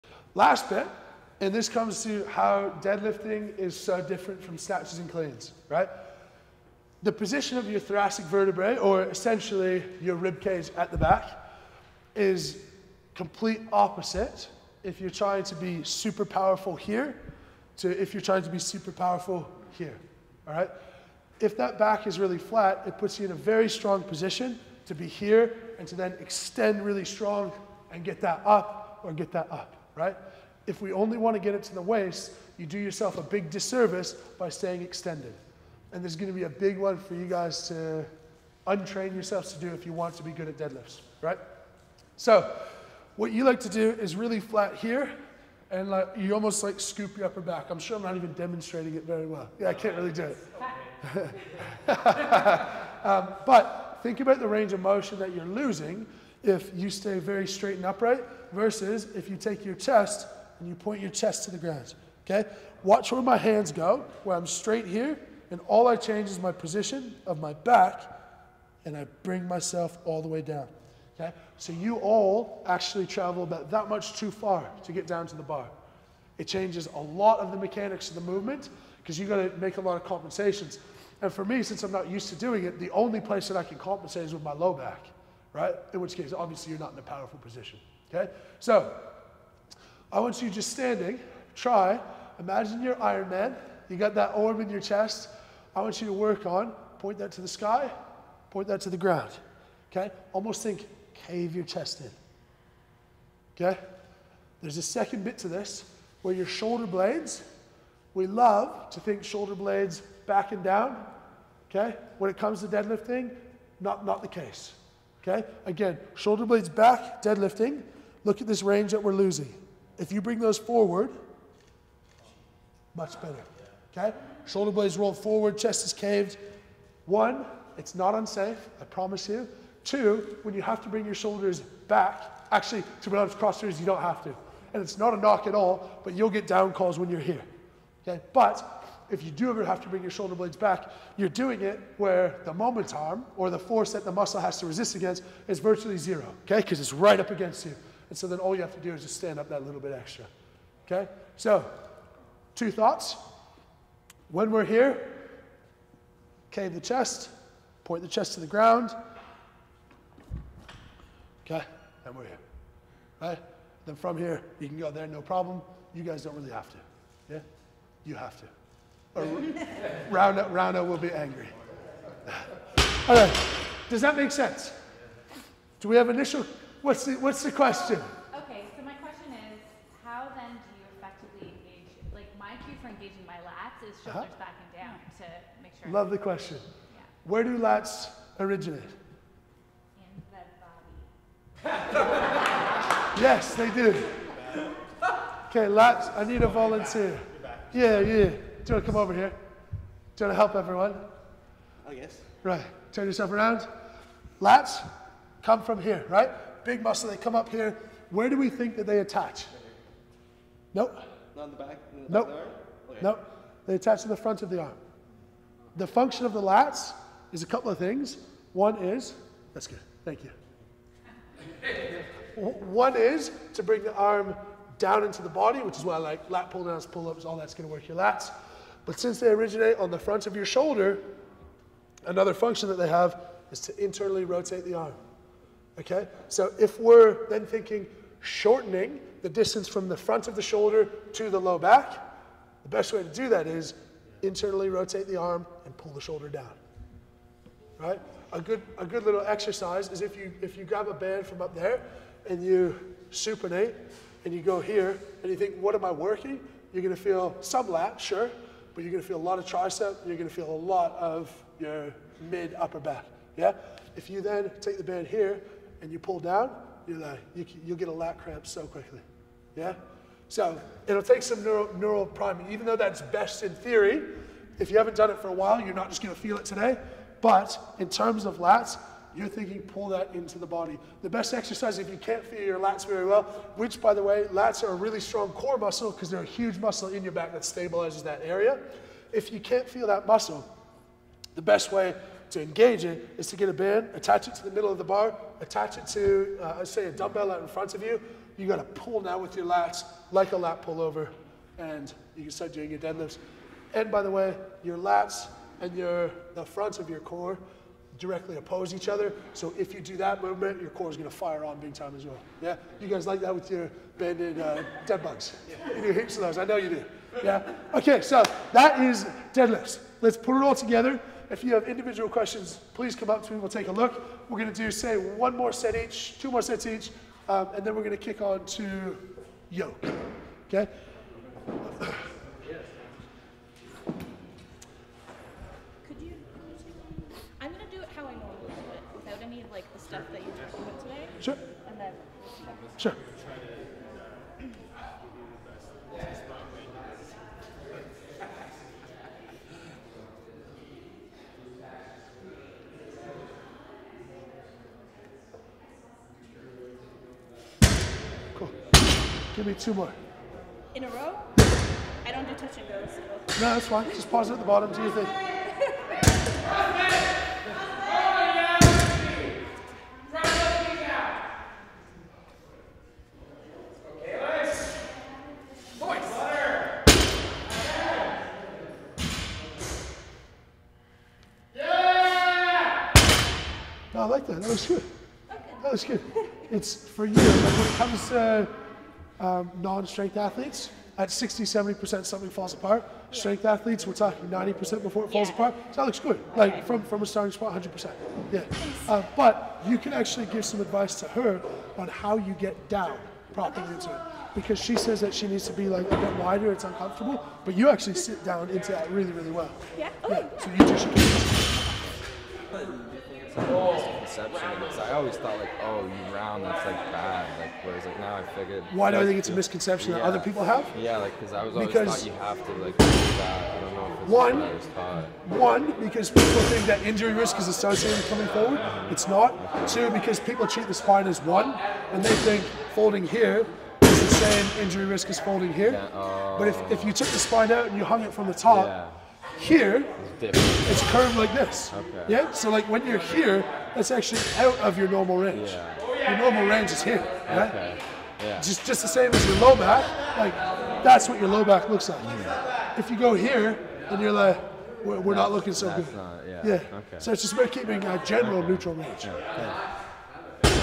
In-Depth Deadlift Thoracic Spine Breakdown - Live Seminar
In this video, I break down what you should be looking for when it comes to your thoracic spine when deadlifting. This video was recorded while I was speaking at a Seminar, so you'll get real questions and examples of the topics discussed.